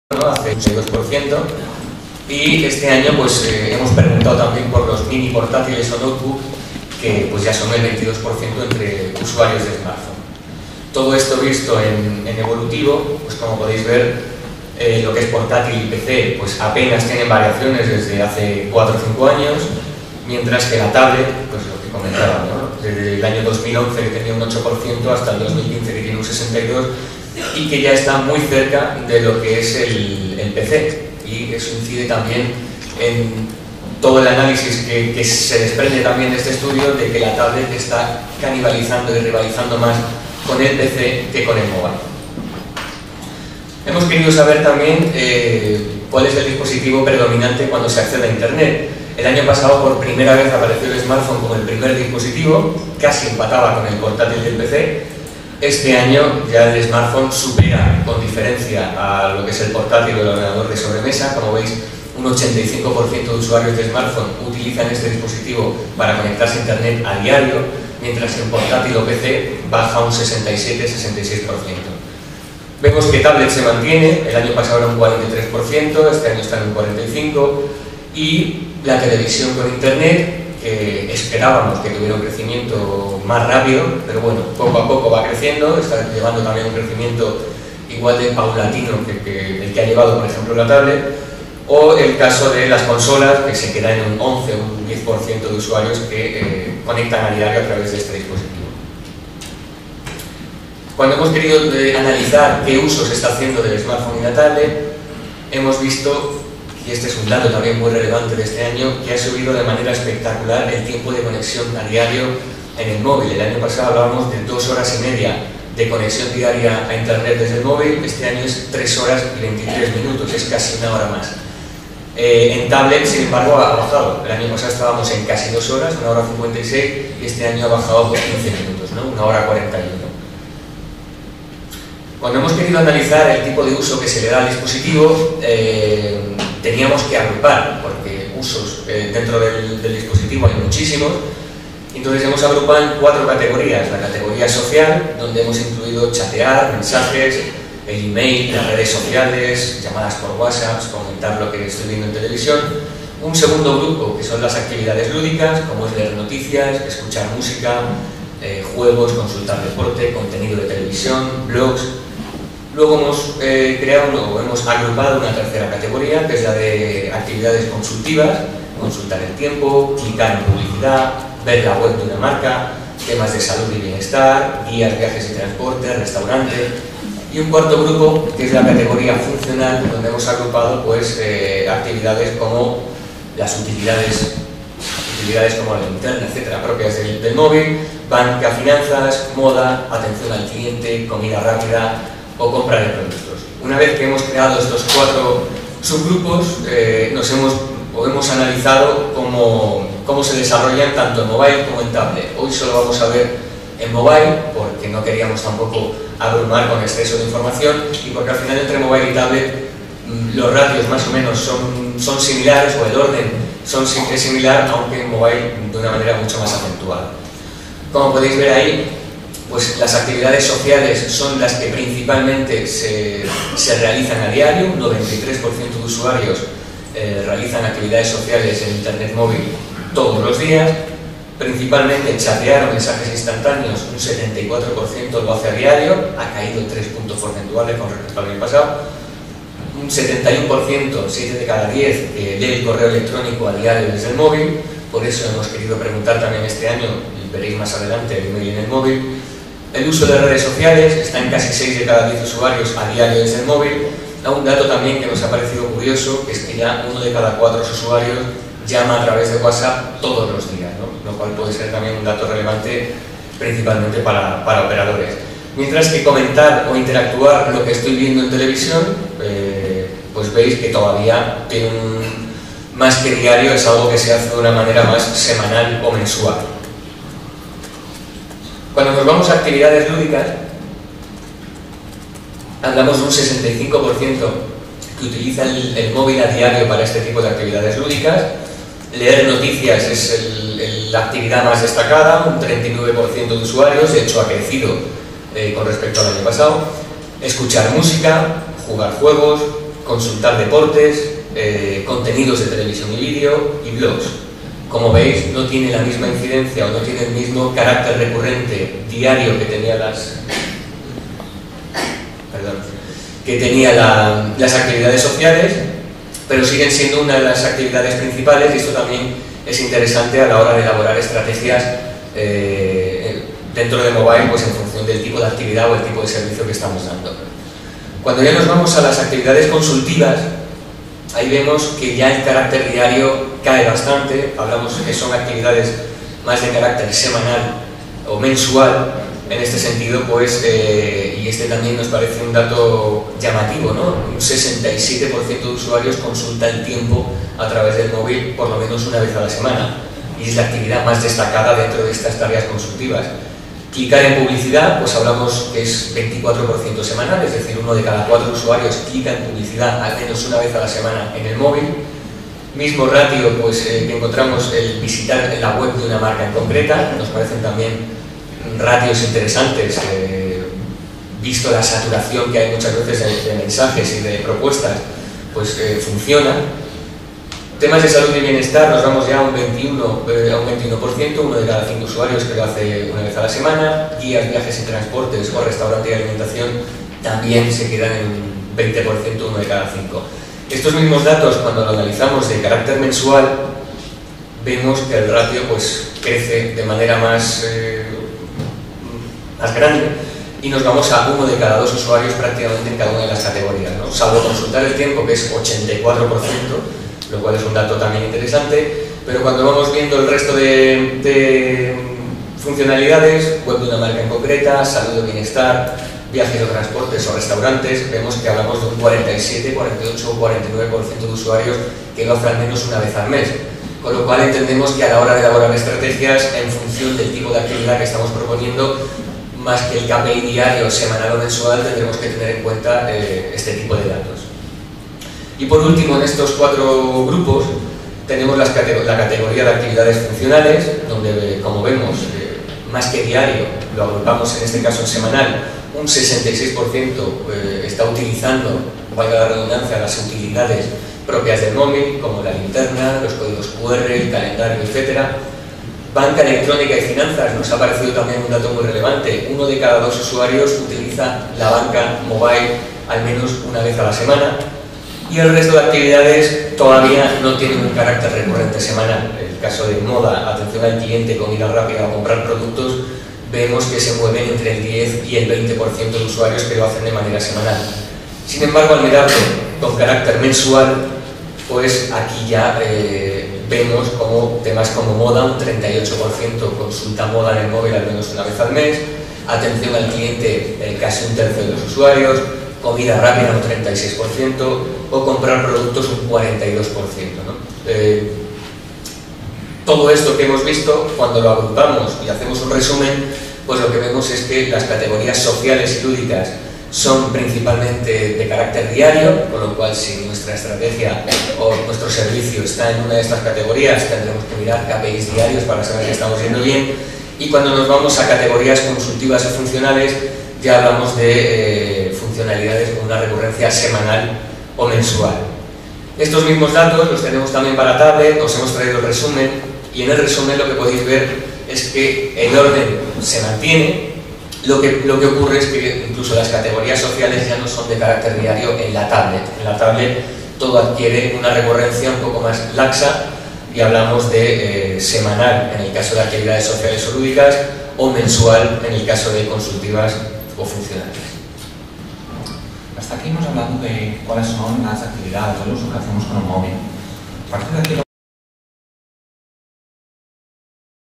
Javier Clarke, Director de Mobile en IAB y Ramón Montanera, Director de VIKO.
Javier Clarke, Director de Mobile New Business & Innovación
Ramón Montanera, Director de Investigación en VIKO, presentan el Estudio Anual de Mobile Marketing 2015 en el que se abordan las tendencias y evolución de los principales indicadores con respecto a anteriores oleadas, la publicidad y herramientas de Mobile Marketing, la percepción de los usuarios y los enfoques de negocio y un avanzar sobre posibles tendencias de negocio en futuro próximo. IABEstudioMobile.

 IAB Spain, la asociación de publicidad, marketing y comunicación digital en España, ha presentado este martes el VII Estudio Anual de Mobile Marketing patrocinado por Repsol y realizado junto con la empresa Elogia. 7 de cada 10 usuarios usa la banca mobile todos los meses. El usuario se conecta a internet móvil 3:23 hrs al día, un aumento de casi 1 hora en un año, principalmente para chatear, uso de las Redes Sociales, eMails y juegos.